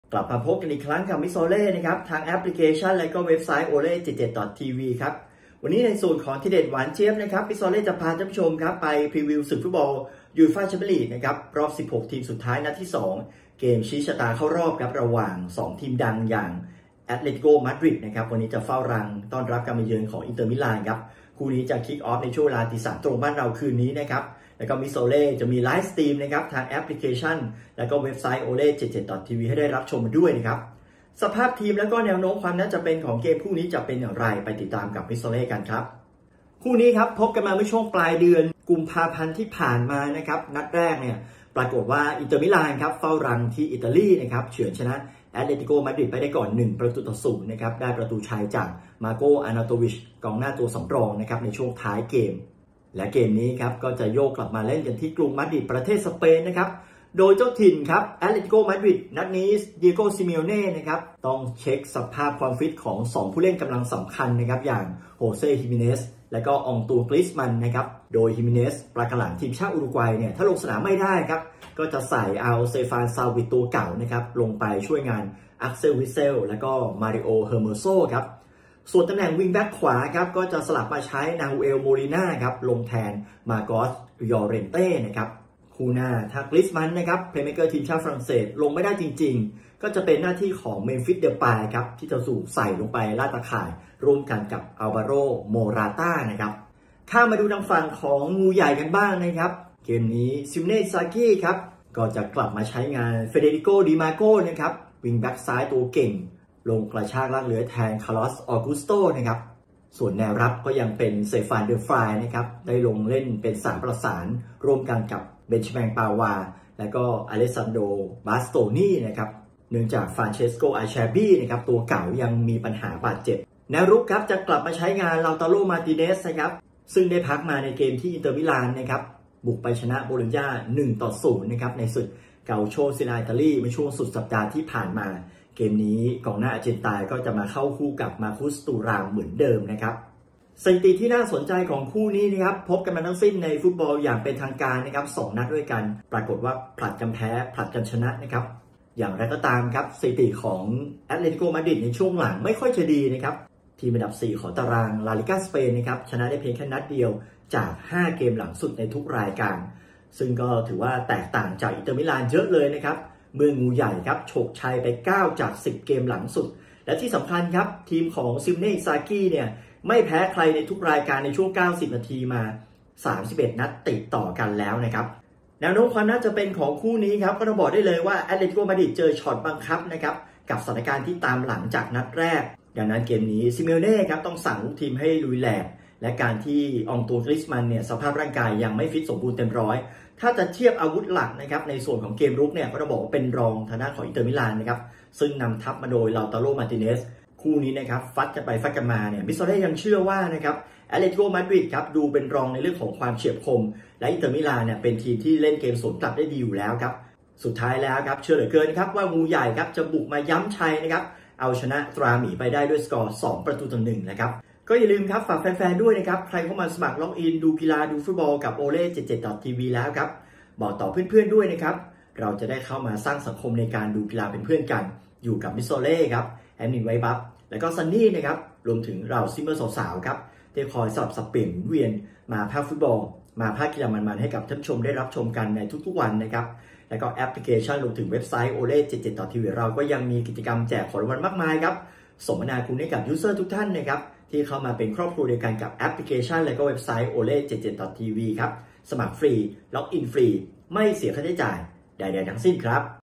ชี้ชะตาเข้ารอบ บิ๊กแมตช์ UCL "ตราหมี" เฝ้าถ้ำพร้อมลุย "งูใหญ่"
ทีเด็ดหวานเจี๊ยบ Mr.Ole พาไปพรีวิวและฟันธงศึกฟุตบอล ยูฟ่า แชมเปี้ยนส์ ลีก รอบน็อกเอาต์ 16 ทีมสุดท้าย นัดที่สอง เกมคู่เอก "ตราหมี" แอตเลติโก้ มาดริด เฝ้าถ้ำรอฉะ "งูใหญ่" อินเตอร์ มิลาน ในคืนวันพุธที่ 13 มีนาคม 2567 เวลา 03.00 น.